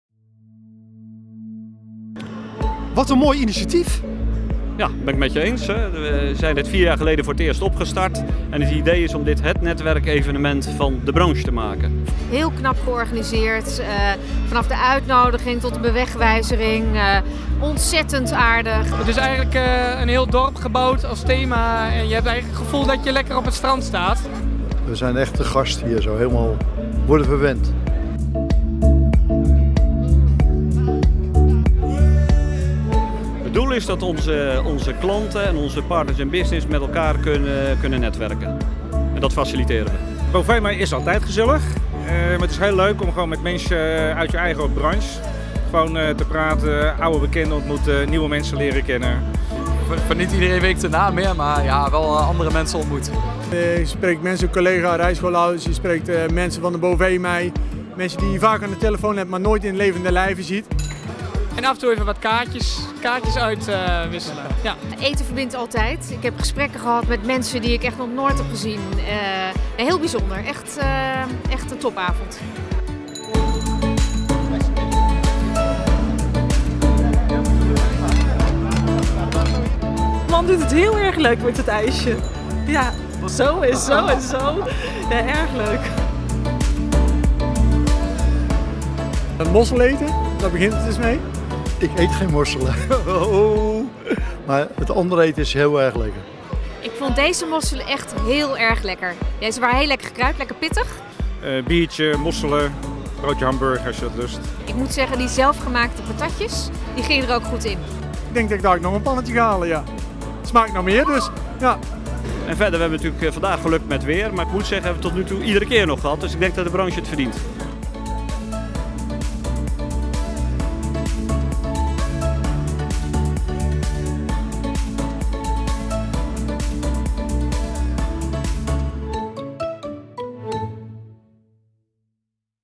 Bovemij Mosselparty 2017
Op donderdag 21 september vond de 4e editie van de Bovemij Mosselparty plaats: het jaarlijkse event waarop we de branche een platform bieden om op een ontspannen manier met elkaar bij te praten en elkaar (beter) te leren kennen.